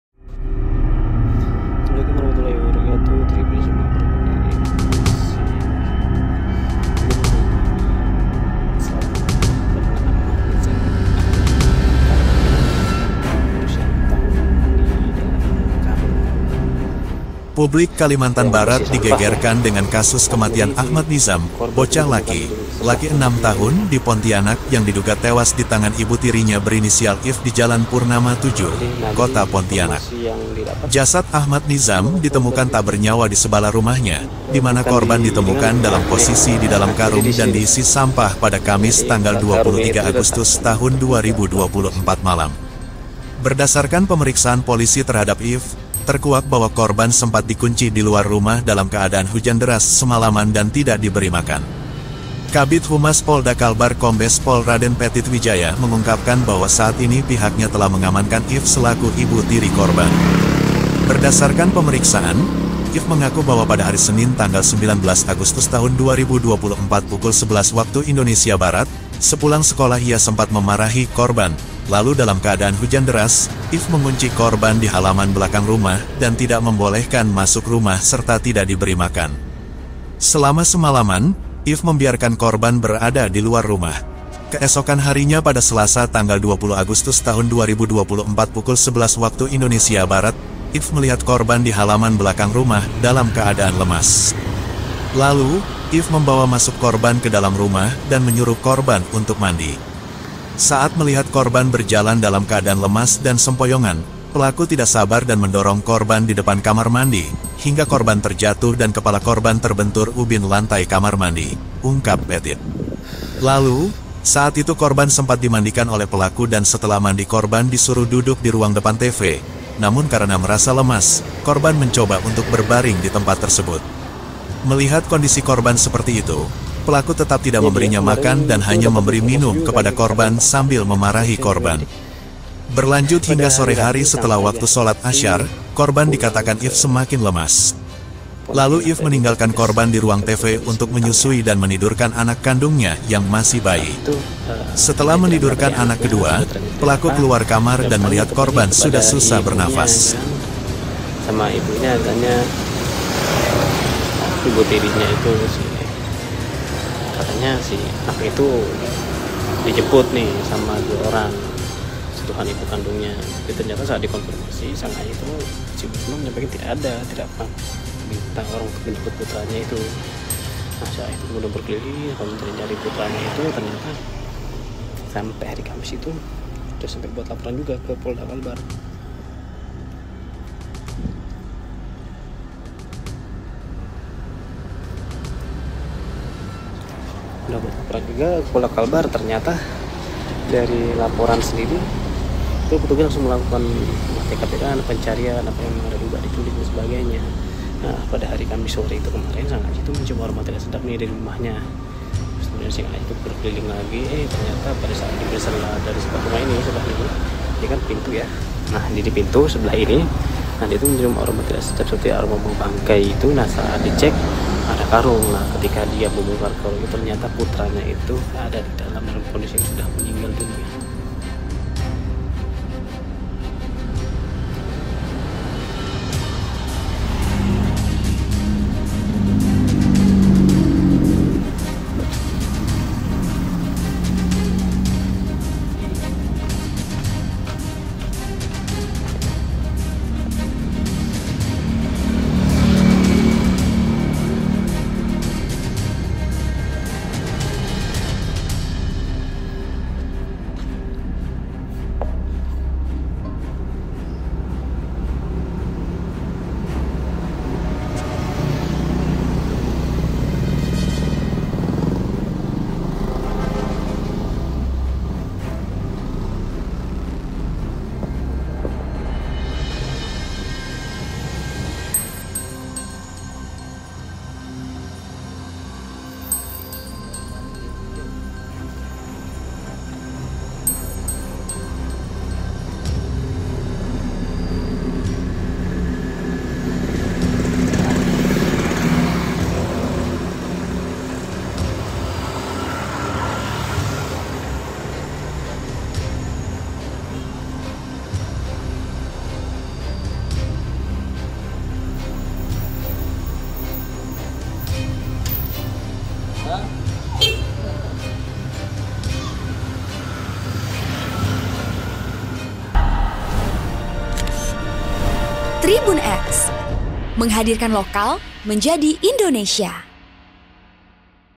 ®️🔴 FAKTA Bocah 6 Tahun Diduga Dibunuh Ibu Tiri❗Dikunci di Luar Rumah Satu Malam & Tak Diberi Makan
Publik Kalimantan Barat digegerkan dengan kasus kematian Ahmad Nizam, bocah laki - laki 6 tahun di Pontianak yang diduga tewas ditangan Ibu Tirinya berinisial IF di jalan Purnama 7, Kota Pontianak.

Jasad Ahmad Nizam ditemukan tak bernyawa di sebelah rumahnya, dimana korban ditemukan dalam posisi di dalam karung dan diisi sampah pada Kamis 23 Agustus 2024 malam.

Berdasarkan pemeriksaan Polisi terhadap IF, terkuak bahwa korban sempat dikunci diluar rumah dalam keadaan hujan deras semalaman dan tidak diberi makan.

Program: -
Host: -
Editor Video: Tito Ramadhani
Uploader: -